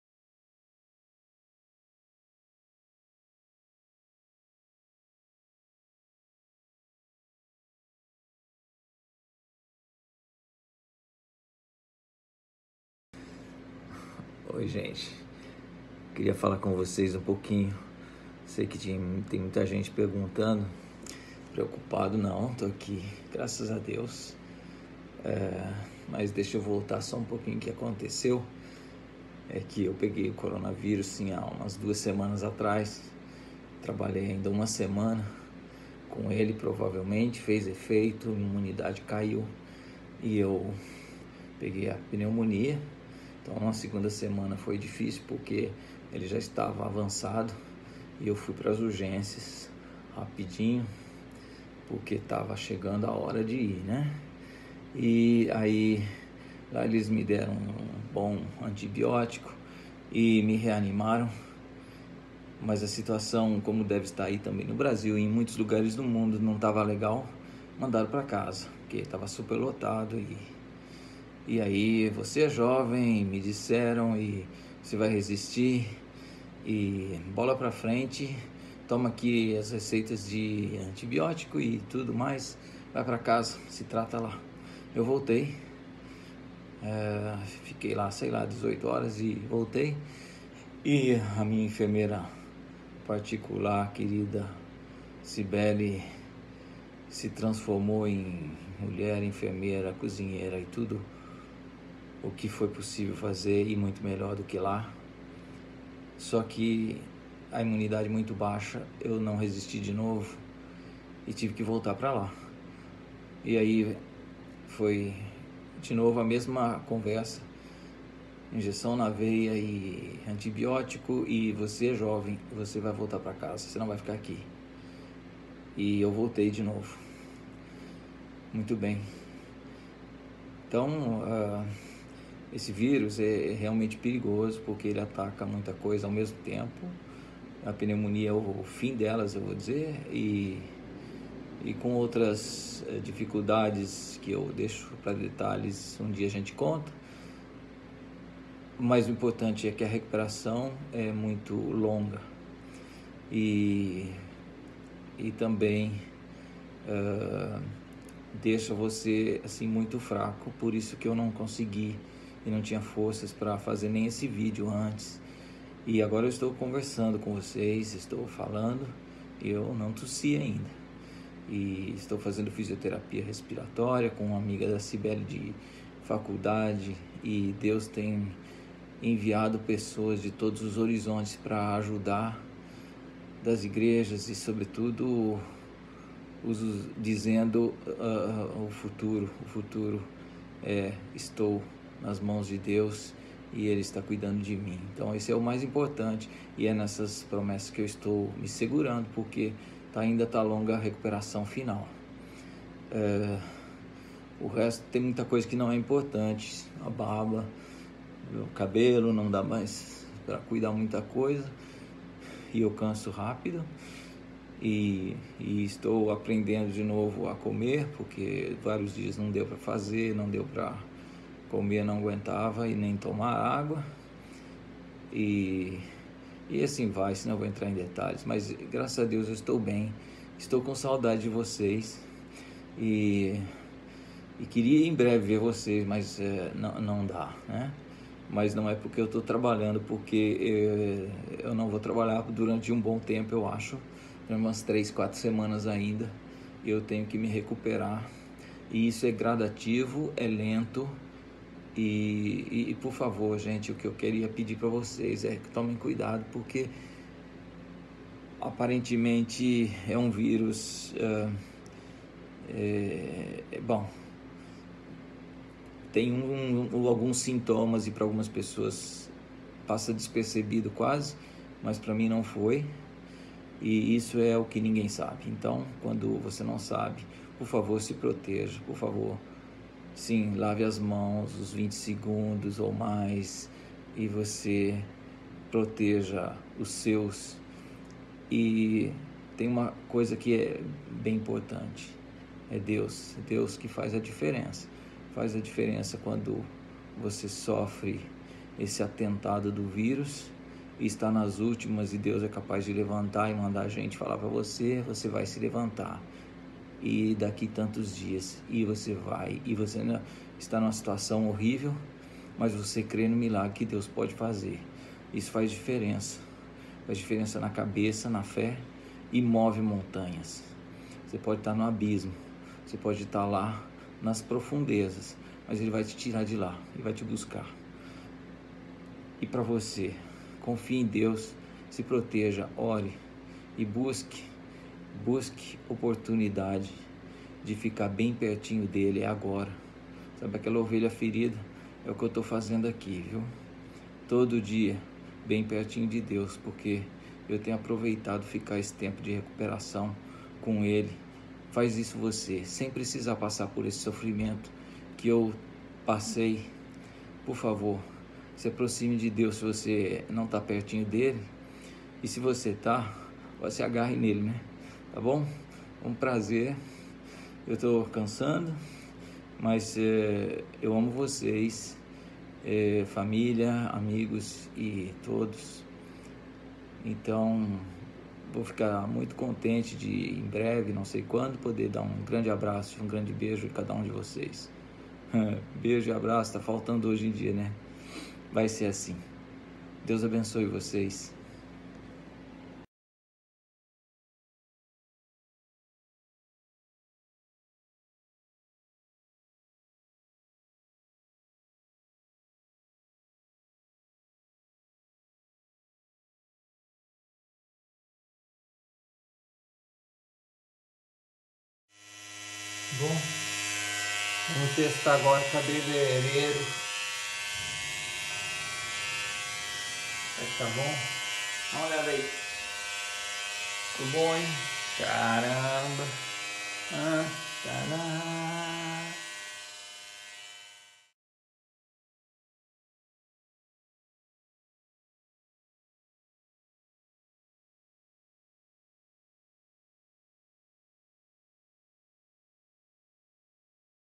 Francisco Campiche e Covid-19
Meu irmão Francisco adquiriu o Covid-19 morando em Jersey City e trabalhando em New York em meados do mês de março de 2020. Graças a Deus, a centenas de orações e o cuidado amoroso de sua esposa, ele segue em recuperação...